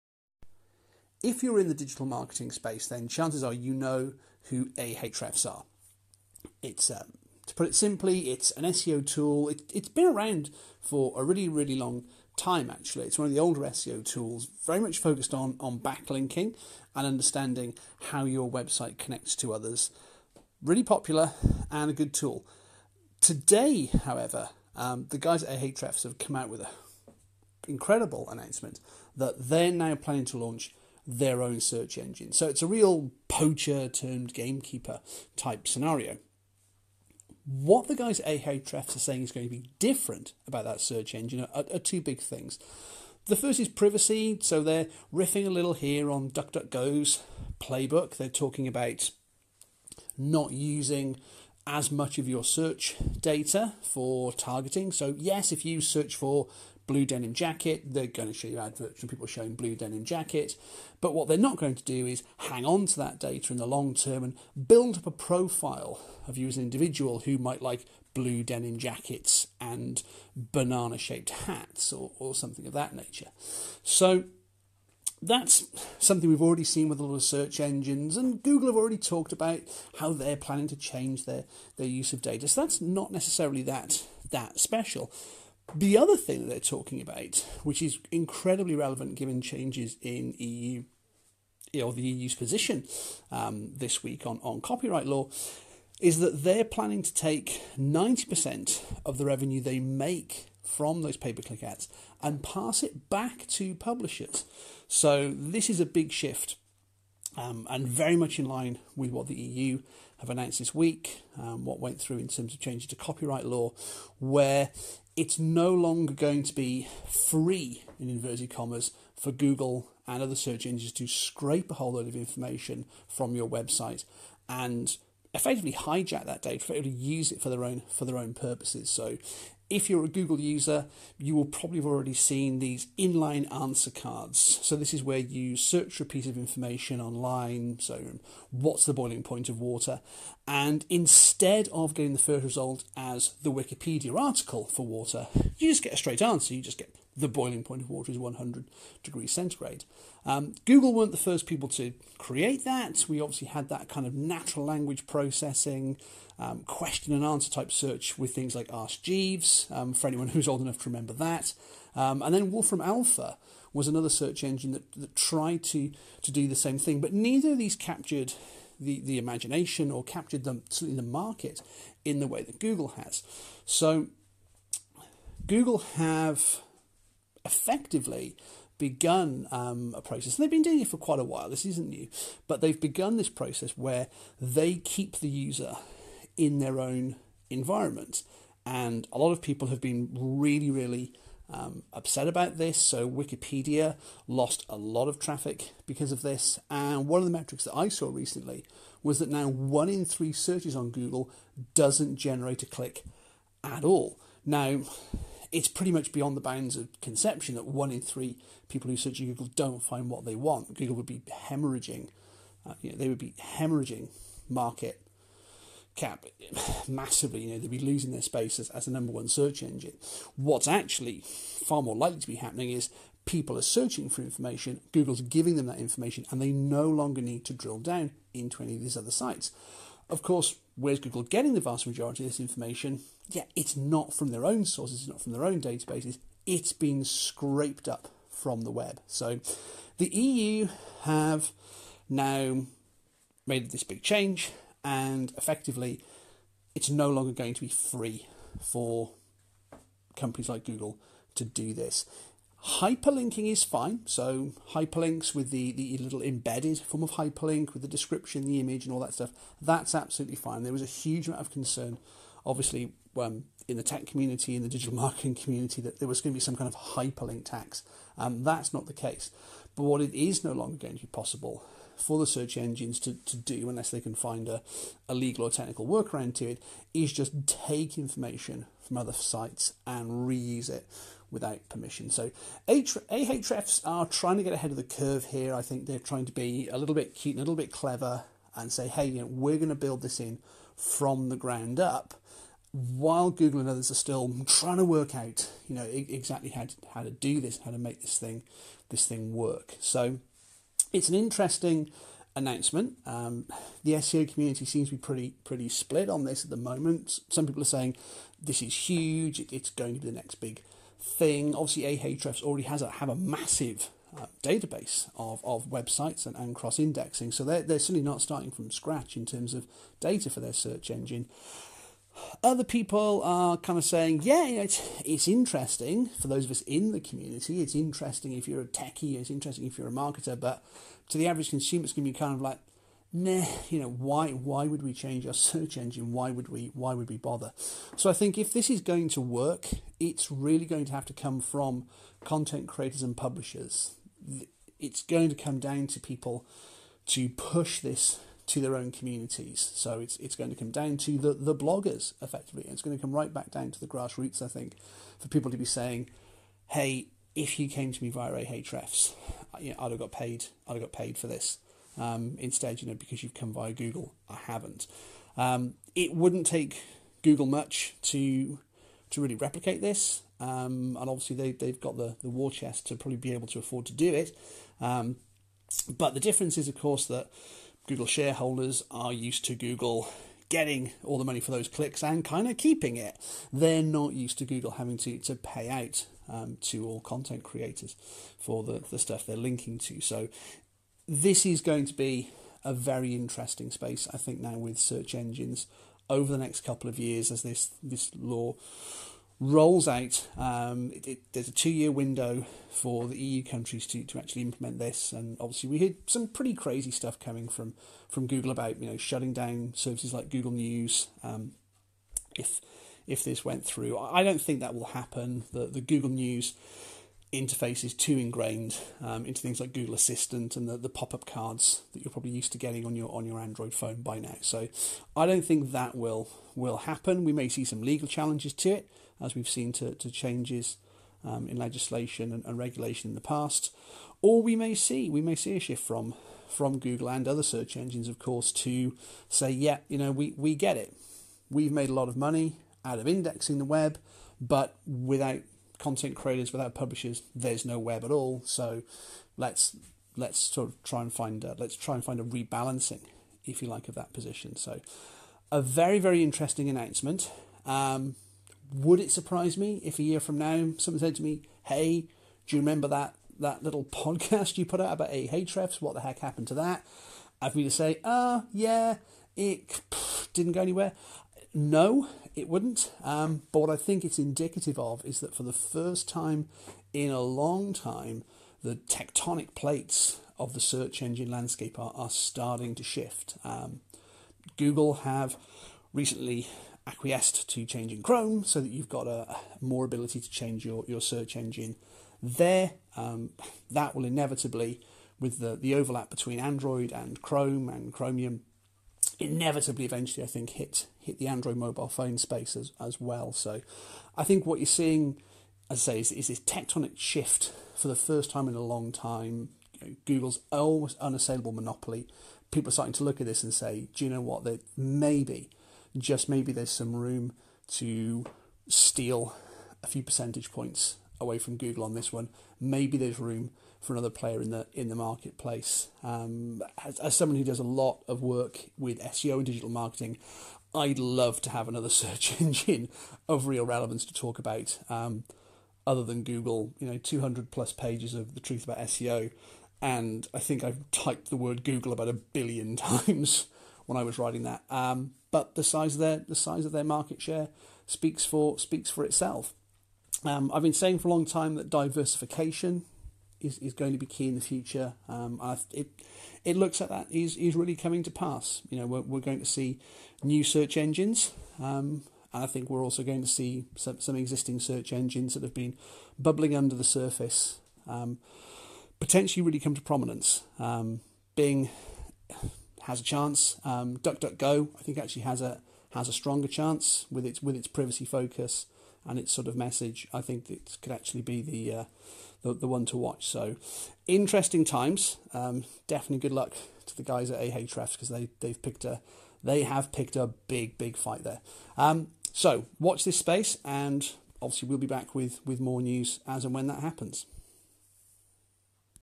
ahrefs launch a search engine. Does it stand a chance?
ahrefs announced plans to launch their own search engine. In a marketplace dominated by Google, does it really stand a chance?